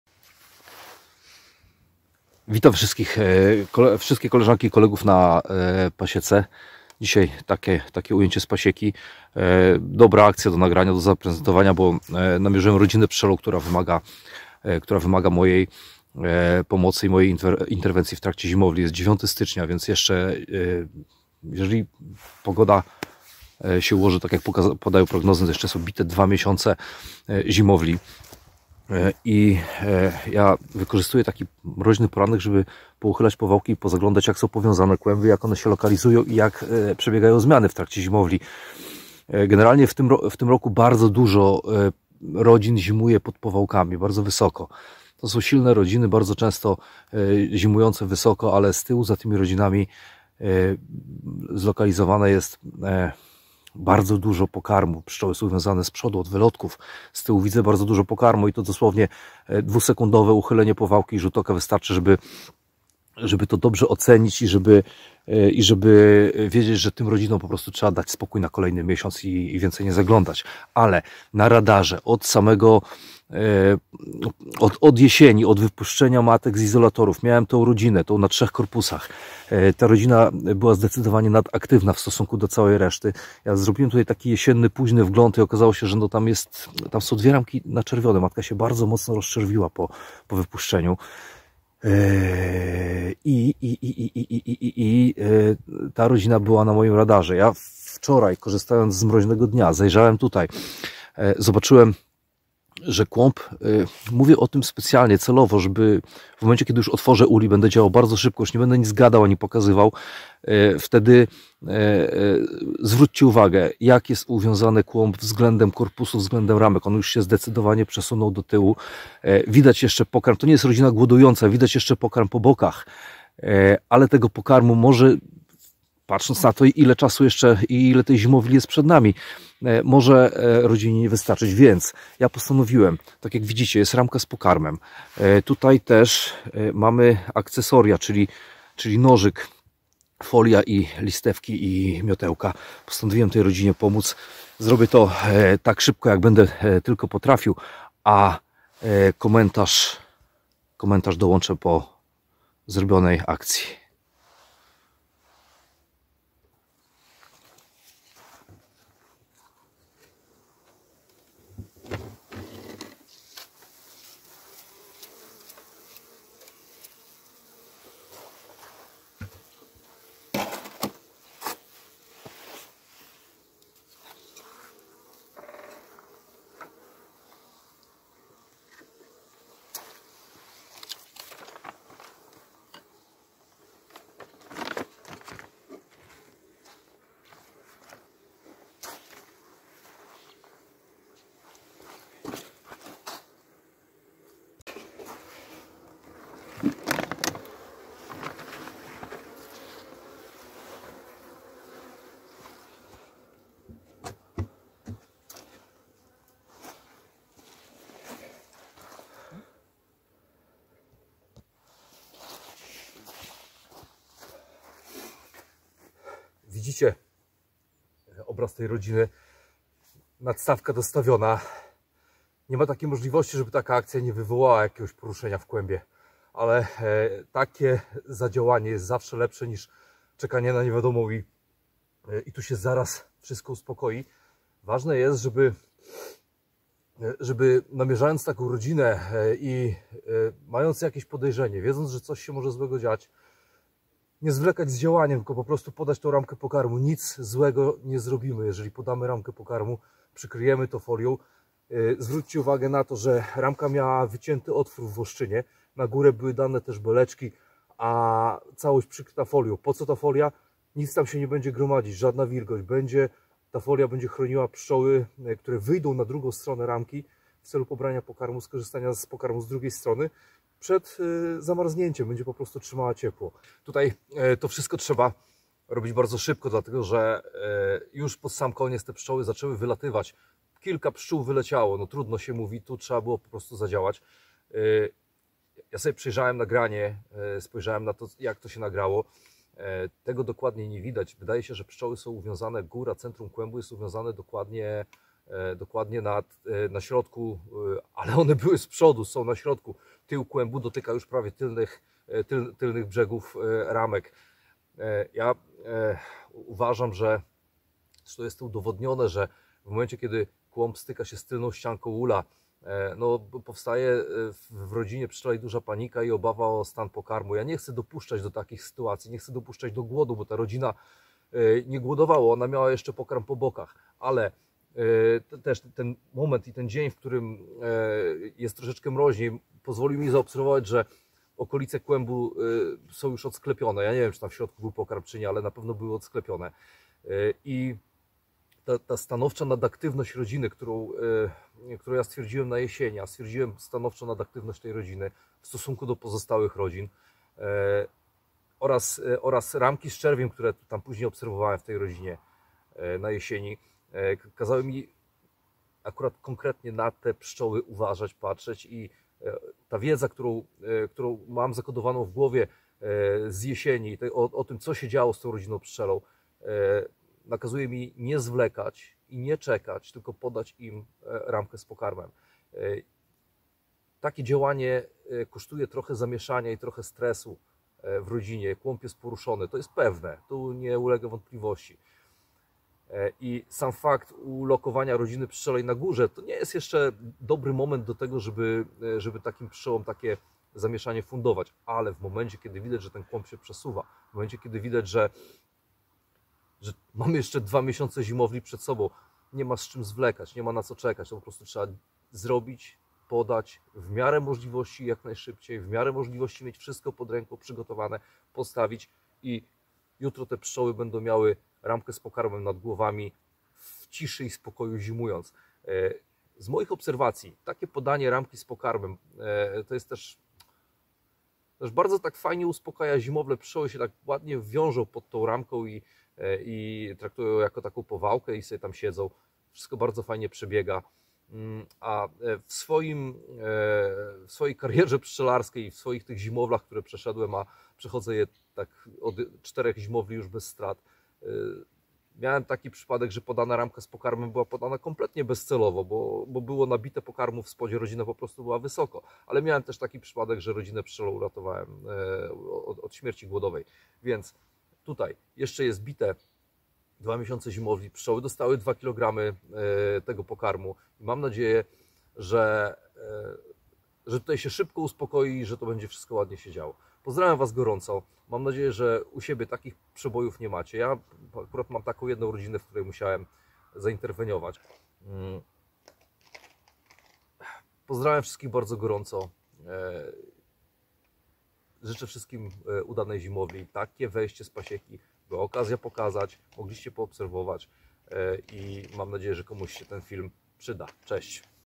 Pomoc rodzinie pszczelej w trakcie zimowli
Obserwacja rodzin w trakcie przygotowań jesiennych, obserwacja przebiegu zimowli,pozwala namierzyć rodziny które potencjalnie mogą mieć problem z pomyślnym przejściem zimy.
Prezentuję pomoc rodzinie która na ten moment jako jedyna na mojej pasiece wymaga mojej interwencji.